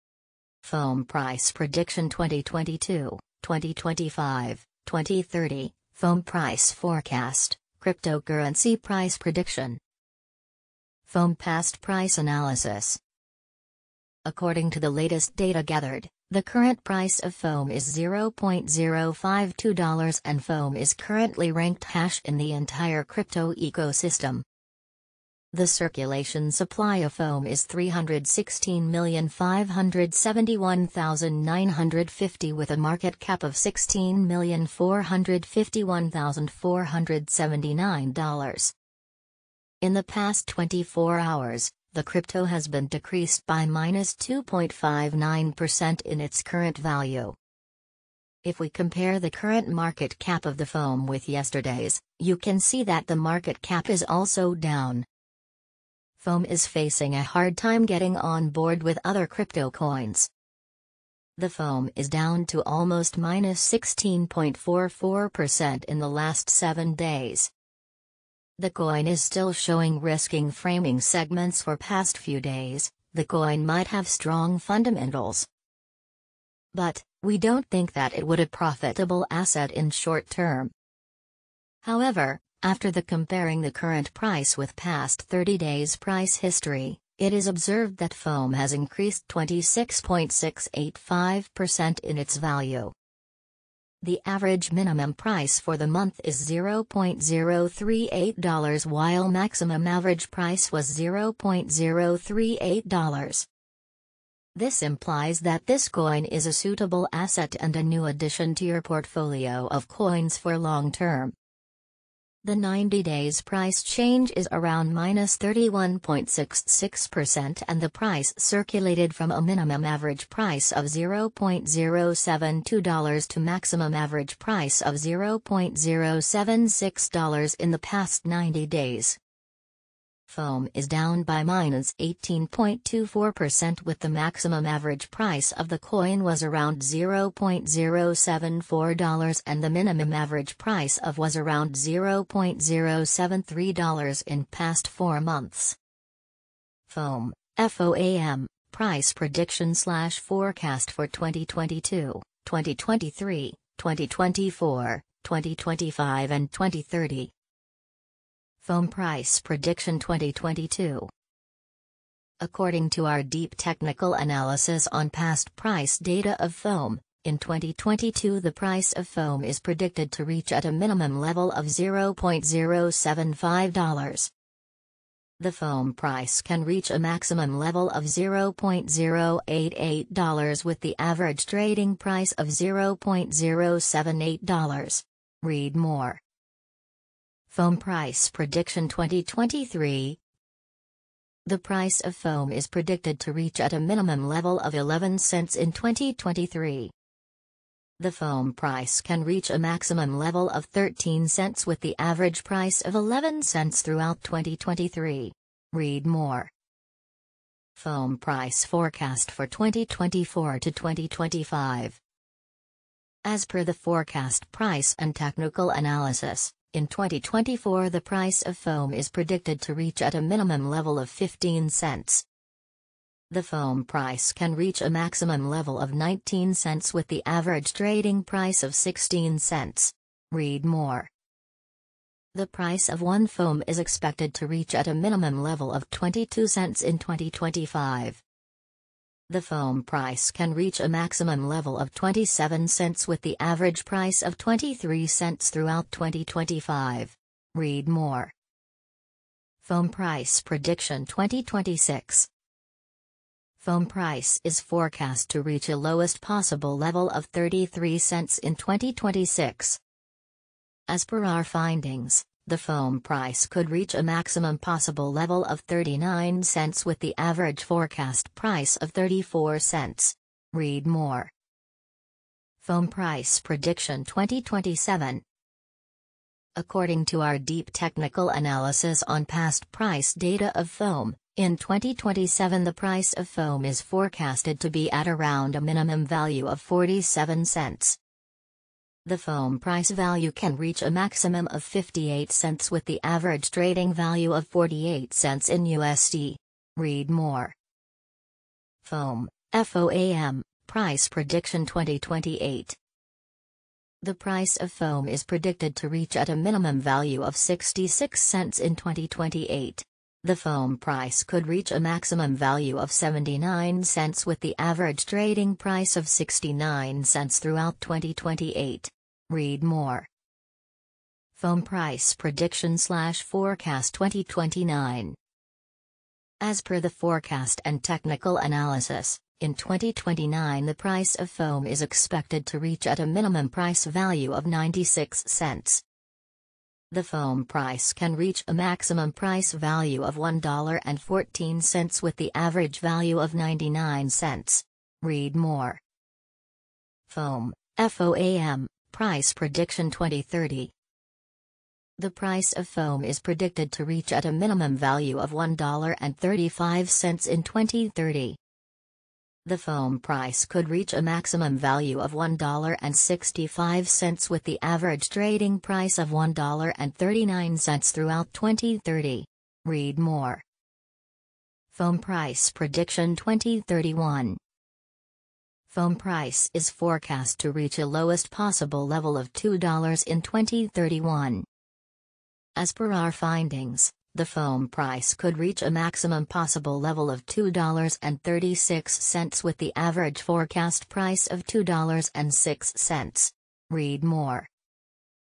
FOAM Price Prediction 2022, 2025, 2030 FOAM Price Forecast Cryptocurrency Price Prediction
FOAM Past Price Analysis
According to the latest data gathered, the current price of FOAM is $0.052 and FOAM is currently ranked # in the entire crypto ecosystem.
The circulation supply of FOAM is 316,571,950 with a marketcap of $16,451,479.
If we compare the current market cap of the FOAM with yesterday’s, you can see that the market cap is also down.
FOAM is facing a hard time getting on board with other crypto coins.
The FOAM is down to almost -16.44% in the last 7 days.
However, after the comparing the current price with past 30 days price history, it is observed that FOAM has increased 26.685% in its value.
The average minimum price for the month is $0.038 while maximum average price was $0.038.
This implies that this coin is a suitable asset and a new addition to your portfolio of coins for long term.
FOAM is down by -18.24% with the maximum average price of the coin was around $0.074 and the minimum average price of was around $0.073 in past 4 months.
FOAM (FOAM) Price Prediction/Forecast for 2022, 2023, 2024, 2025 and 2030
FOAM Price Prediction 2022
According to our deep technical analysis on past price data of FOAM, In 2022 the price of FOAM is predicted to reach at a minimum level of $0.075.
FOAM Price Prediction 2023
The price of FOAM is predicted to reach at a minimum level of $0.11 in 2023.
The FOAM price can reach a maximum level of $0.13 with the average price of $0.11 throughout 2023.
FOAM Price Forecast for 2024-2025
As per the forecast price and technical analysis, In 2024 the price of FOAM is predicted to reach at a minimum level of $0.15.
The price of 1 FOAM is expected to reach at a minimum level of $0.22 in 2025.
The FOAM price can reach a maximum level of $0.27 with the average price of $0.23 throughout 2025.
FOAM Price Prediction 2026
FOAM price is forecast to reach a lowest possible level of $0.33 in 2026.
FOAM Price Prediction 2027
According to our deep technical analysis on past price data of FOAM, In 2027 the price of FOAM is forecasted to be at around a minimum value of $0.47.
The FOAM price value can reach a maximum of $0.58 with the average trading value of $0.48 in USD.
FOAM (FOAM) Price Prediction 2028
The price of FOAM is predicted to reach at a minimum value of $0.66 in 2028. The FOAM price could reach a maximum value of $0.79 with the average trading price of $0.69 throughout 2028.
FOAM Price Prediction/Forecast 2029
As per the forecast and technical analysis, In 2029 the price of FOAM is expected to reach at a minimum price value of $0.96.
FOAM (FOAM) Price Prediction 2030
The price of FOAM is predicted to reach at a minimum value of $1.35 in 2030.
The FOAM price could reach a maximum value of $1.65 with the average trading price of $1.39 throughout 2030.
FOAM Price Prediction 2031
FOAM price is forecast to reach a lowest possible level of $2.00 in 2031.

MORE :
Cryptocurrency Price Prediction